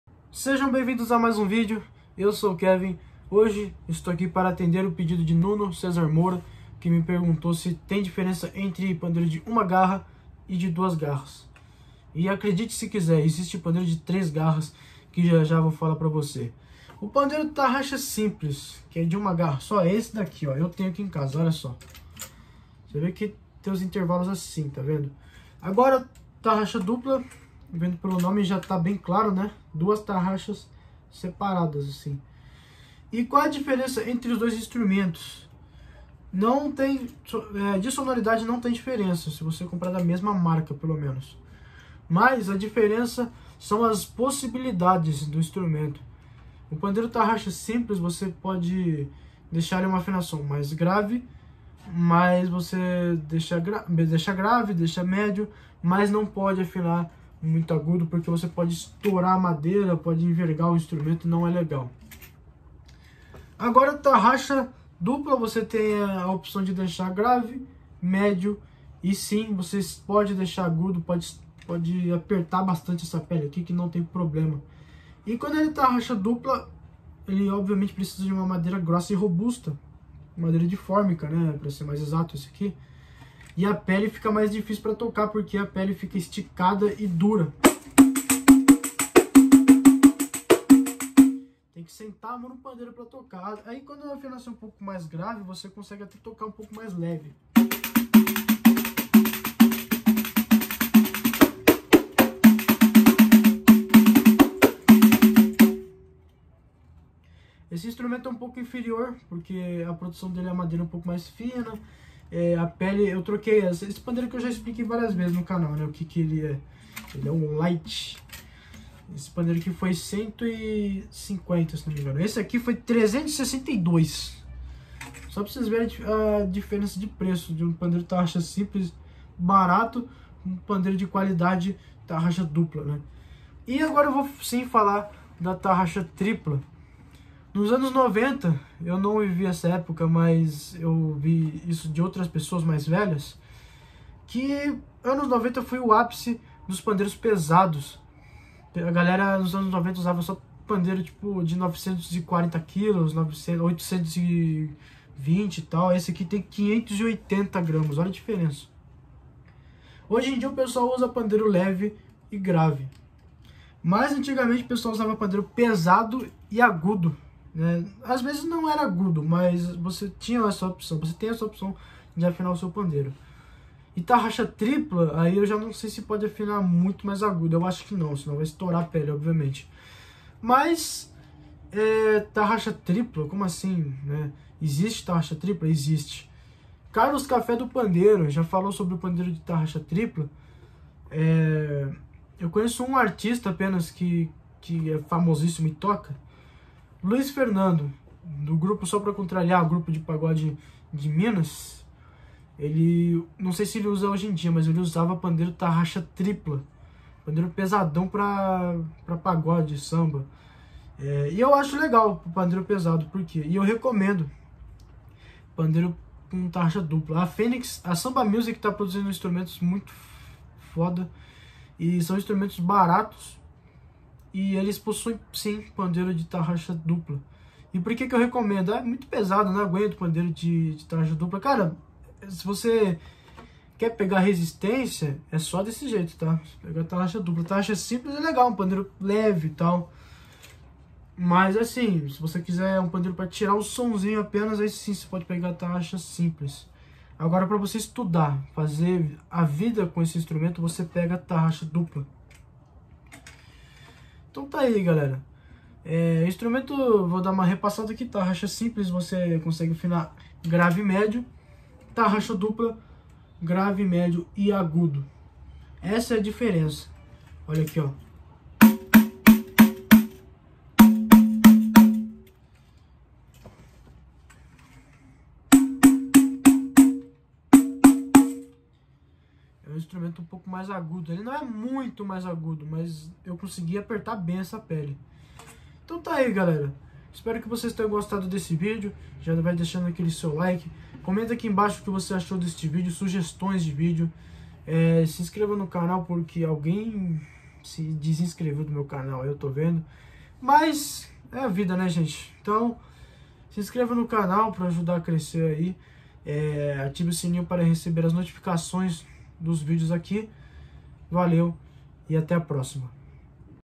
PANDEIRO TARRAXA SIMPLES E DUPLA
Qual será a diferença entre os pandeiros de uma garra e duas garras de afinação? Confira o vídeo acima.